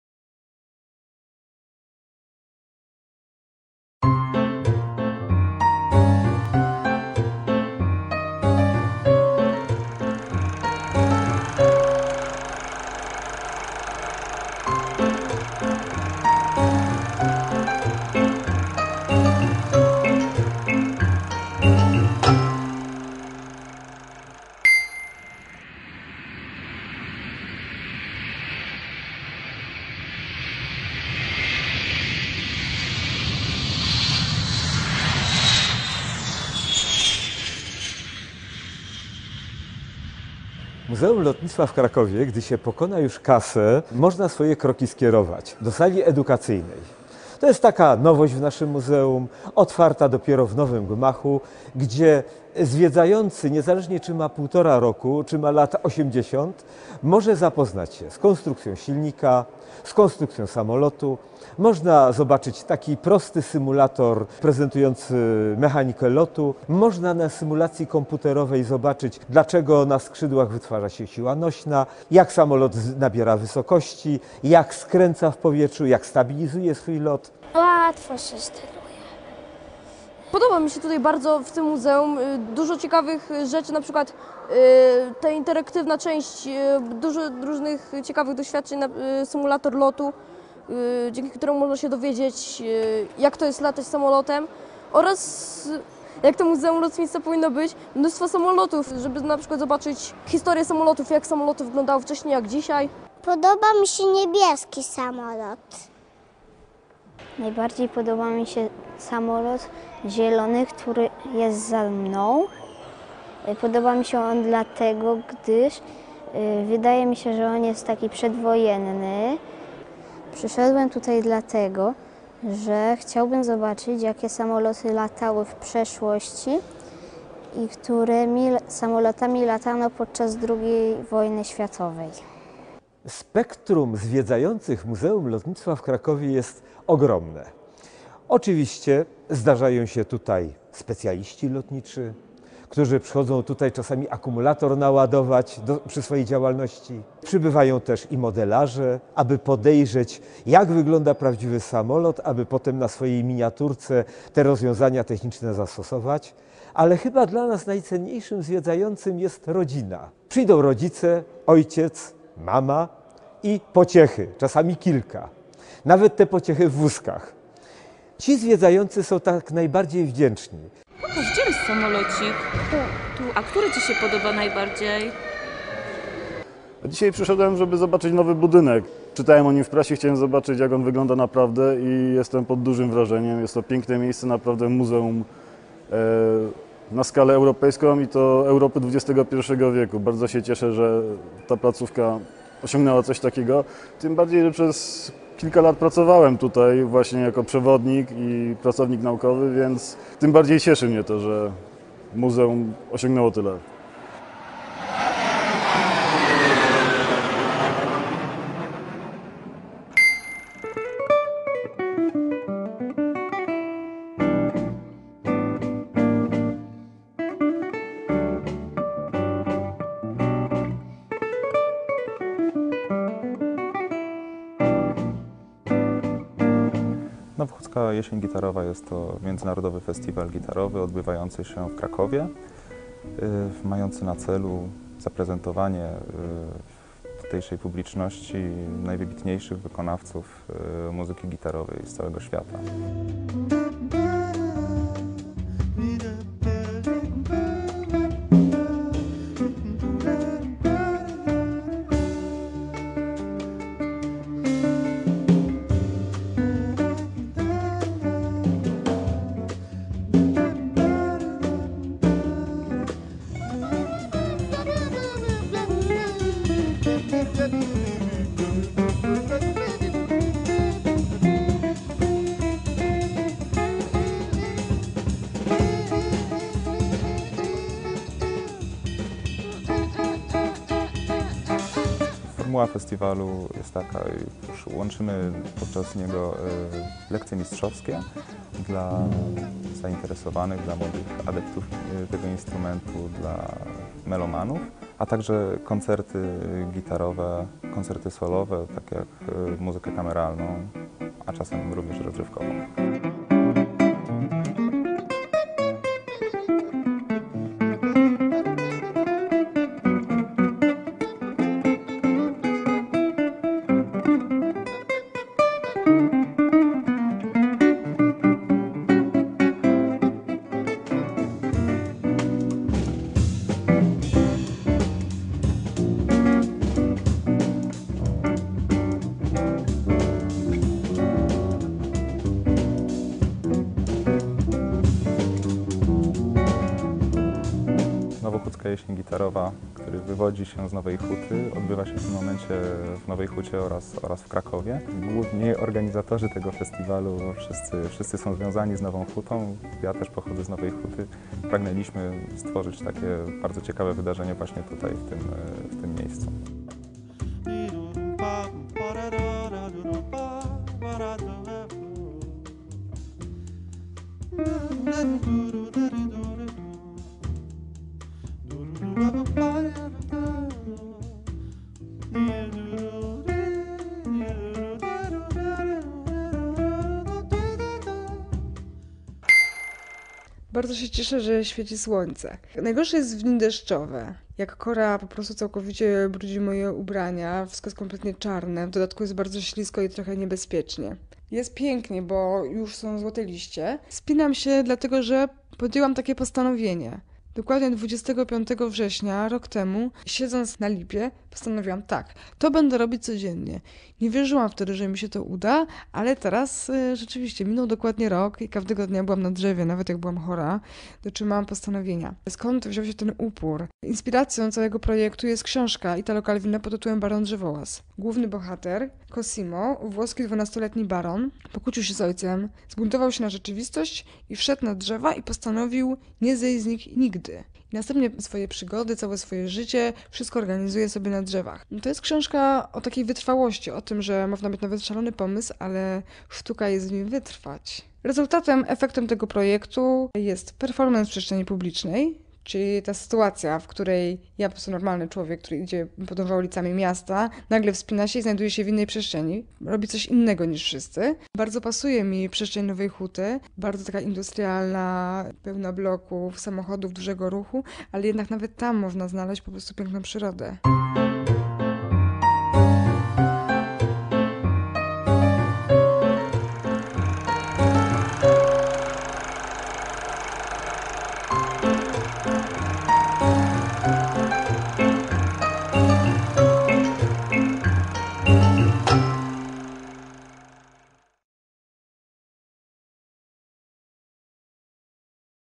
Nowohucka Kronika Filmowa nr 223
NOWY GMACH
W Muzeum Lotnictwa Polskiego w Krakowie otwarto właśnie nowy gmach. Najważniejsza w nim jest – jak podkreśla Jan Hoffman – SALA EDUKACYJNA. A najmilej widzianymi gośćmi – całe rodziny. Tu zwiedzający w absolutnie każdym wieku mogą poznać konstrukcję silnika, samolotu, skorzystać z prostych symulatorów lotu, zrozumieć jakie siły oddziałują na lecącą maszynę, obejrzeć autentyczne mundury i pamiątki. Swoimi wrażeniami dzielą się z nami młodzi i najmłodsi: Stanisław Skowroński (docenia interaktywność muzeum), Szymon (najbardziej lubi ten niebieski samolot) i Krystian (nie ma to jak przedwojenne maszyny).

GITAROWA UCZTA
Nowohucka Jesień Gitarowa to międzynarodowy festiwal, którego celem jest prezentacja najwybitniejszych wykonawców muzyki gitarowej. Oprócz koncertów w różnych miejscach Krakowa, odbywają się również lekcje mistrzowskie. Reportaż prezentuje fragmenty koncertu inauguracyjnego, który odbył się na deskach Teatru Ludowego. Gwiazdą wieczoru była grupa Alieksey Vianna Trio.
O festiwalu opowiada jego Dyrektor Artystyczny Roch Modrzejewski.

SPOTKANI W NOWEJ HUCIE: CECYLIA MALIK – PERFORMERKA
Performerka Cecylia Malik, realizując fragment swojego artystycznego projektu "365 drzew" na terenie Nowej Huty, opowiada jednocześnie, jak to się stało, że dokładnie 25 września 2009r, siedząc na lipie, postanowiła: będę to robić codziennie! Inspiracją do jej umieszczonego w przestrzeni publicznej performancu była książka "Baron drzewołaz" Italo Calvino.